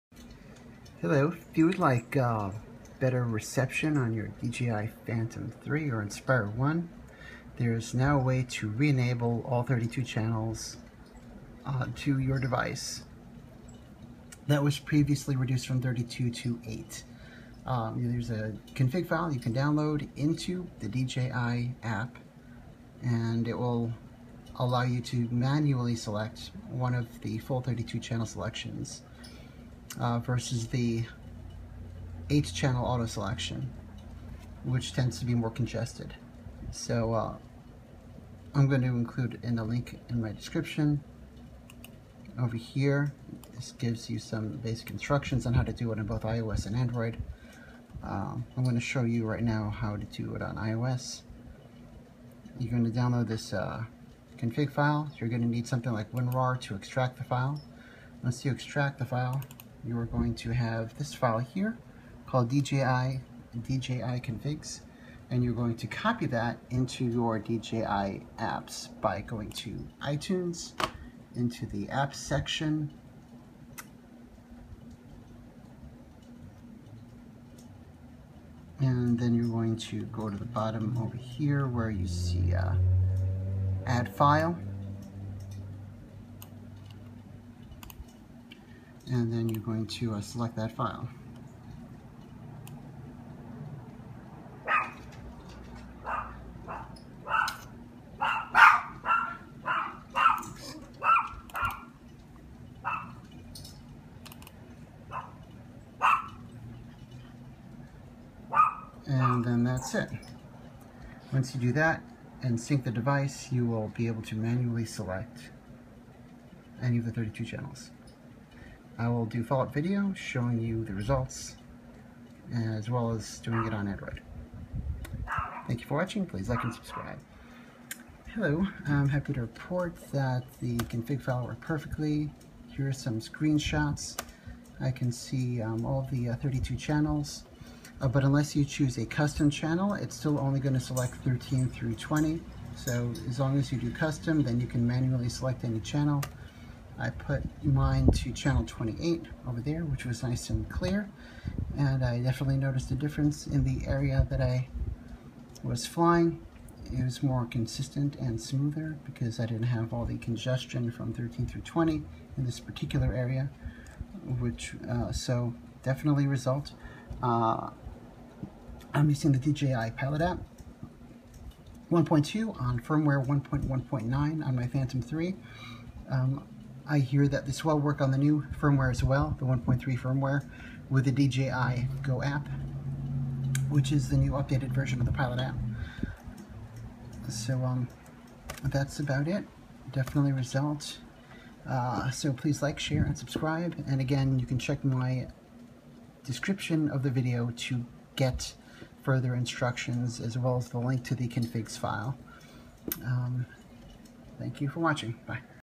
How To Enable 32 Channels on Phantom 3 4 Inspire 1 for your Video and Control FPV
How to enable the full 32 channels back for your DJI Phantom 3 and Inspire 1 quad copters.  I also heard this works on the Phantom 4 though I don't have one myself to test that.  You will need to download a config file into the app and do a manual channel selection.  Works for both iOS and Android on both the DJI Pilot app 1.2 and DJI GO

*Note* if the "custom" channel selection item is greyed out, which is required to take advantage of all 32 channels.    You will need to change your regional settings. Here's a video with info on it this here.

Here is an updated link location to download the config file. It is also now a ZIP file which makes things simpler.

note:  MAC OS users need to set their system to show hidden files or you will not be able to find the file.